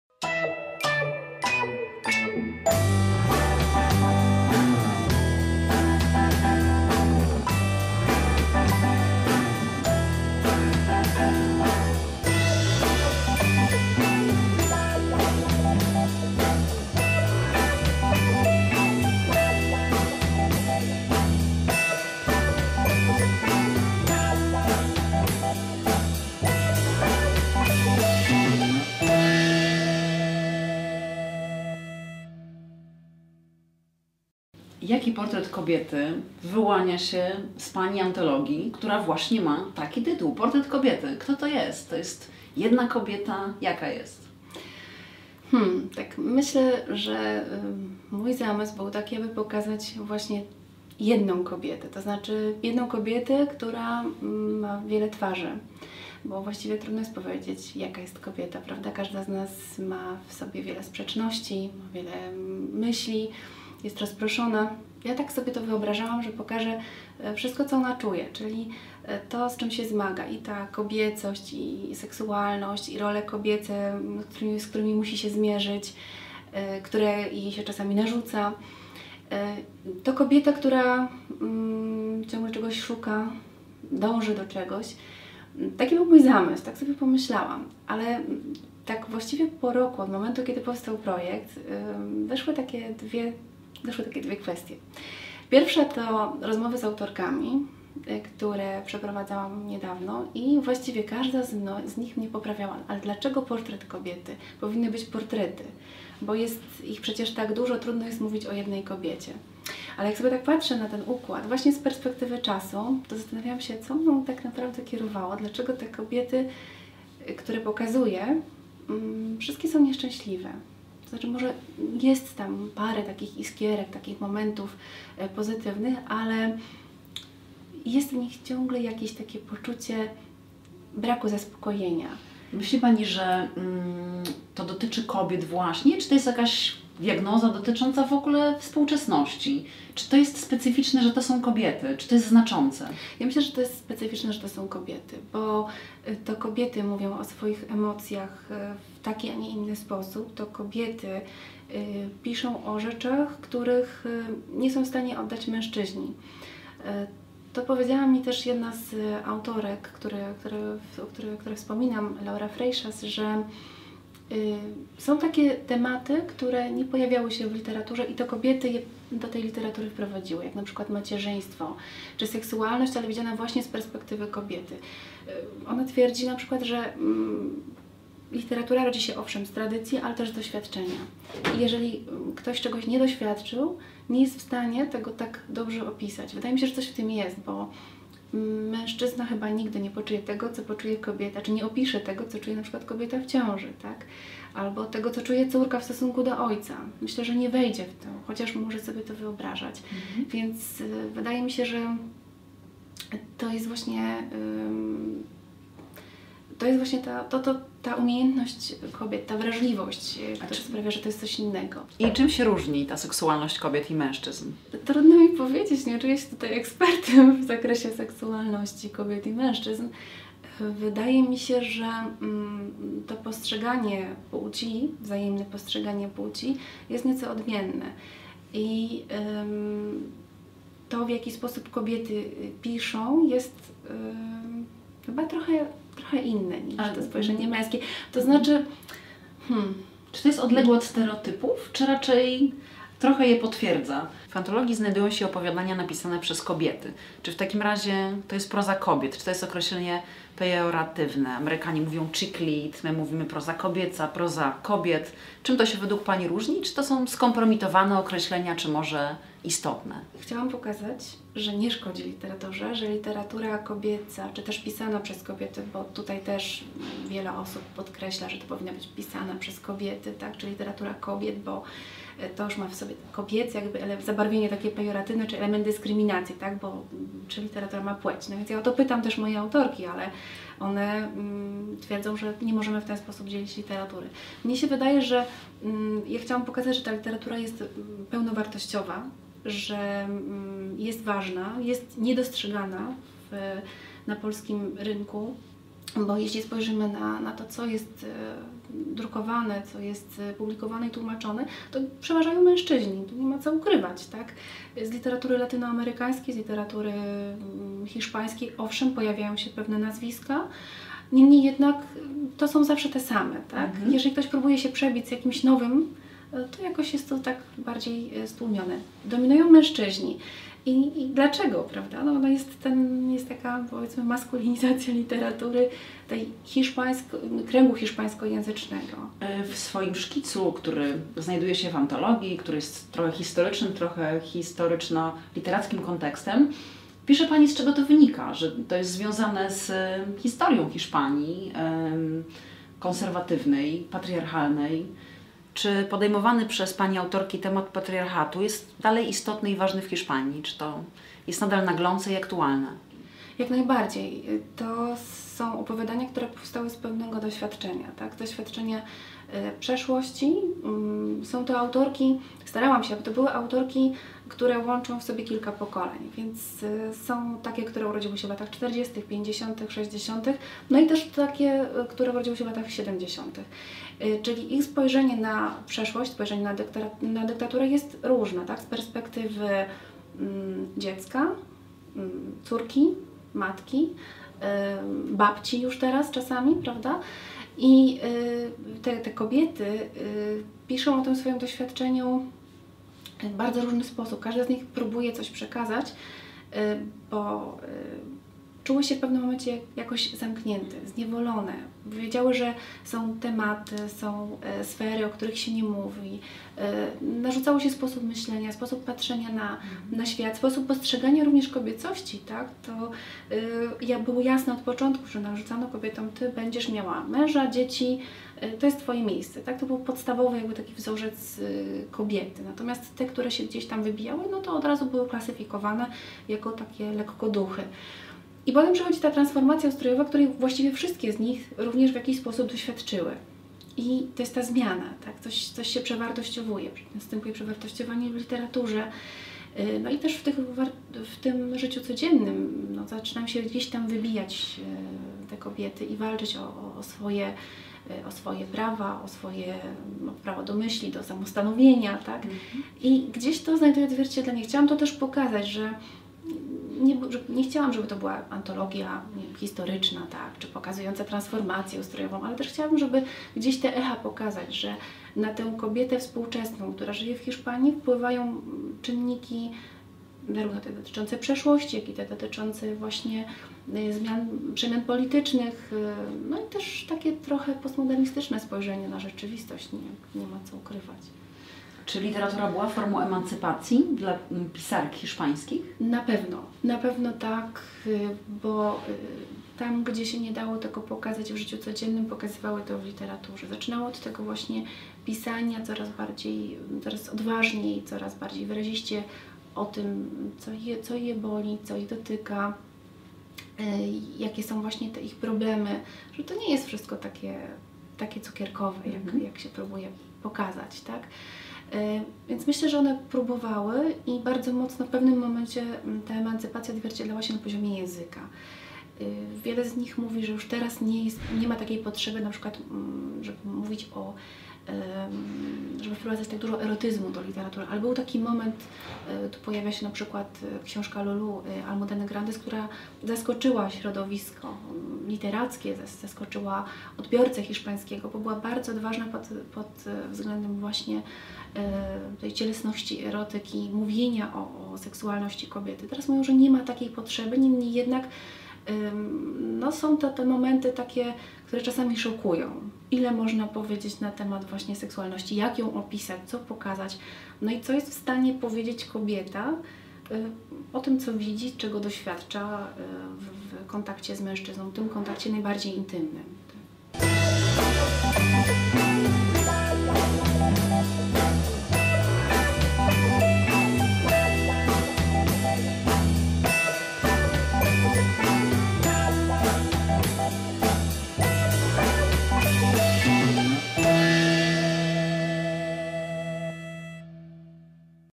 Między książkami: Małgorzata Kolankowska, część 1
Pierwsza część rozmowy Agnieszki Wolny-Hamkało z Małgorzatą Kolankowską autorką wyboru i przekładu antologii "Portret kobiety. W opowiadaniach dziesięciu hiszpańskich autorek".